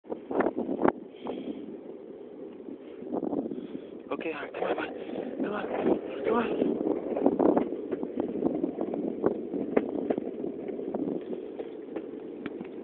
Harley Our MInPin Chihuahua mix loves to go run along side the longboard
LongBoarding with Harley Minpin chihuahua mix (CHIPIN)